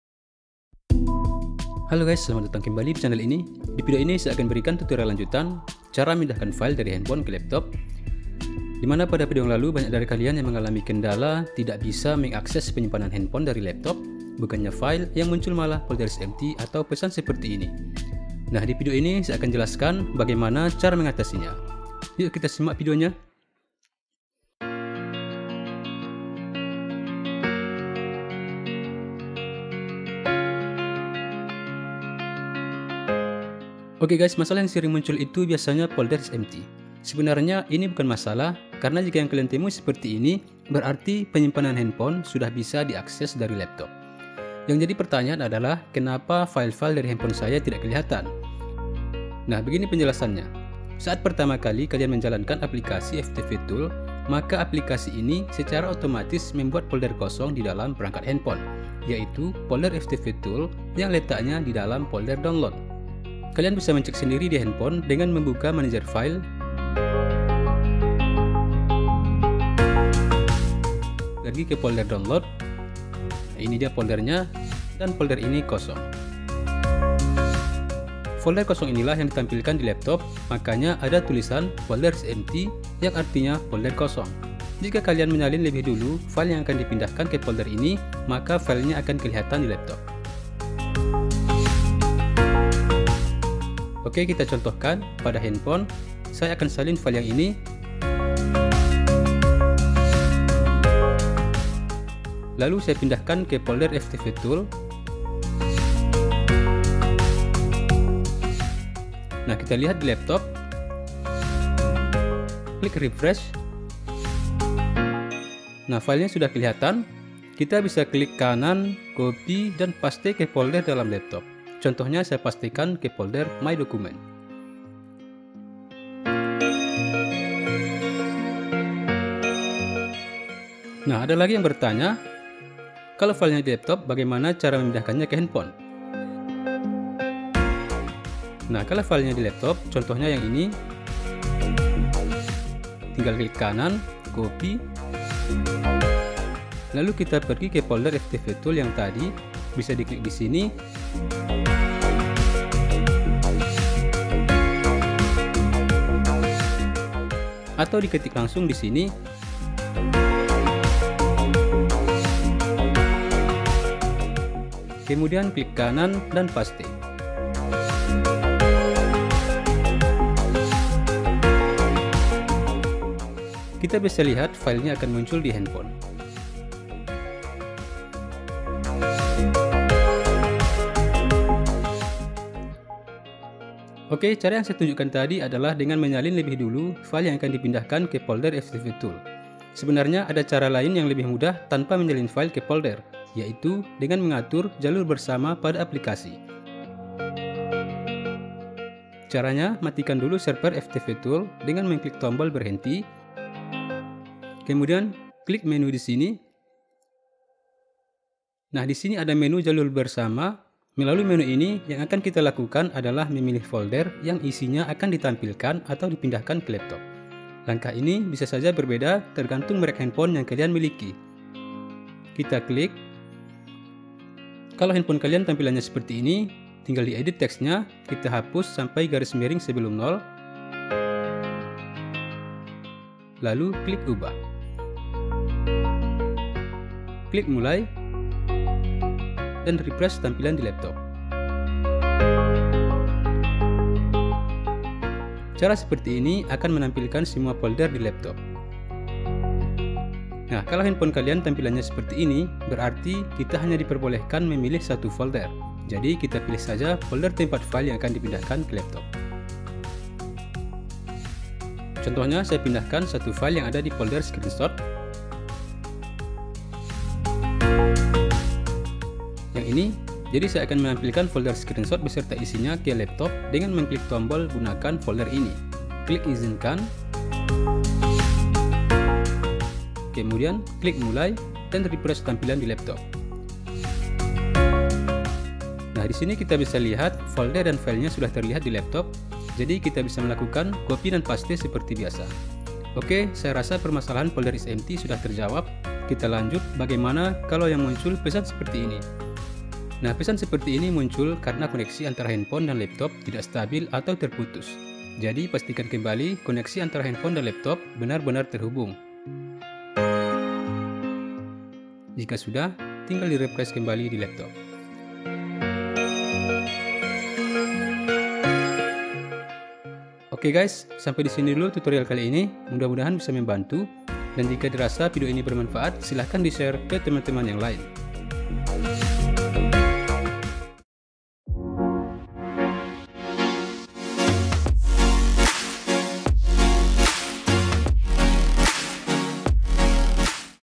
UPDATE!! Cara Memindahkan File dari HP ke Laptop Tanpa Kabel Data
Video lanjutan tentang Cara Memindahkan File dari HP ke Laptop Tanpa Kabel Data.